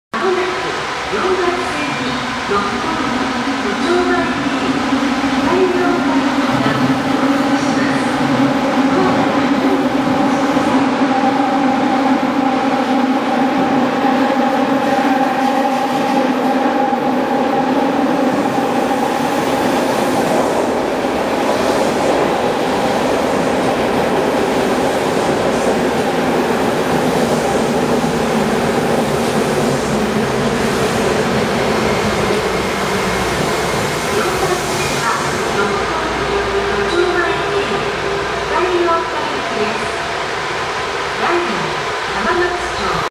都営大江戸線 12-000形 普通列車 大門駅入線
2018年9月23日に撮影。
都営地下鉄 12-000形8両
都営大江戸線  普通列車
大門駅入線シーン